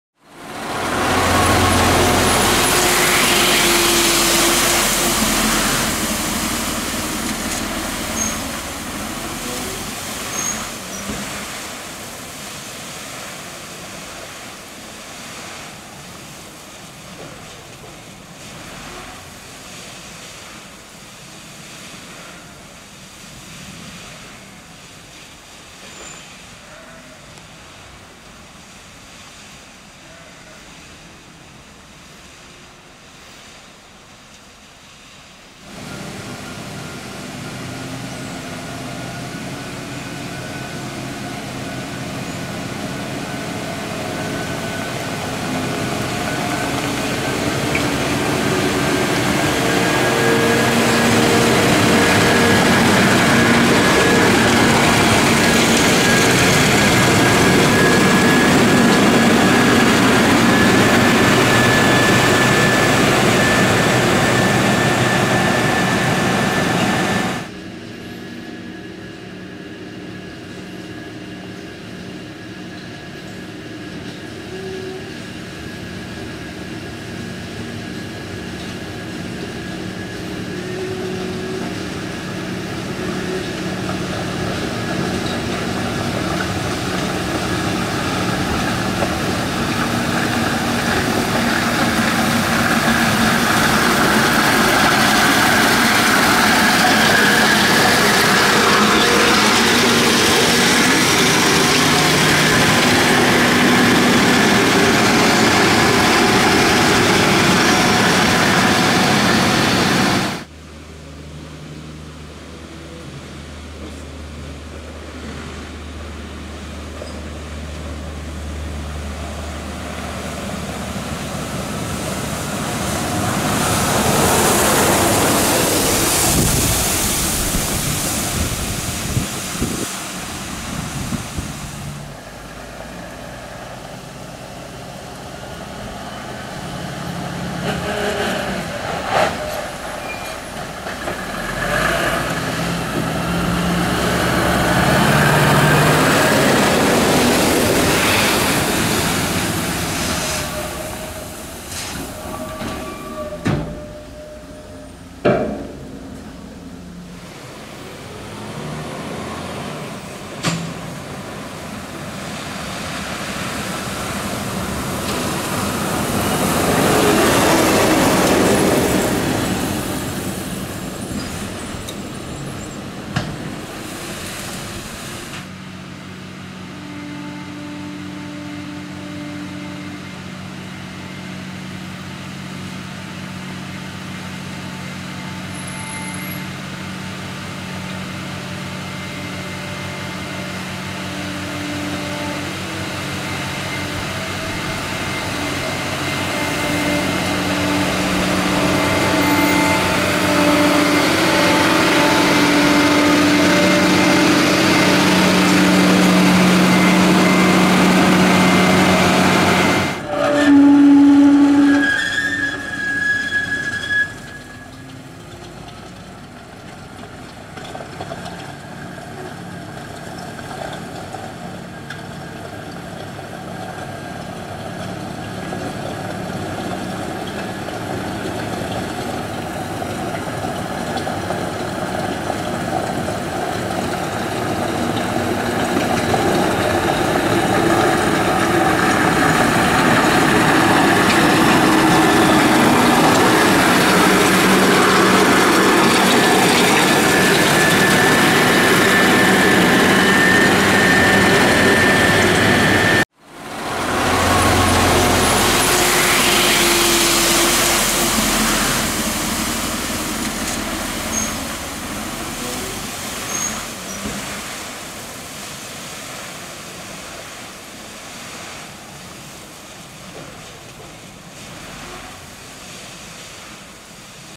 Mobil Tank Tempur Sedang Perang Di Pantai | Kobelco Mobil Tank Mainan
Mobil Tank Tempur Sedang Perang Di Pantai  Kobelco Mobil Tank Mainan anak anak dan mobilan

sedang bertempur Mobil Tank Tempur dan mainan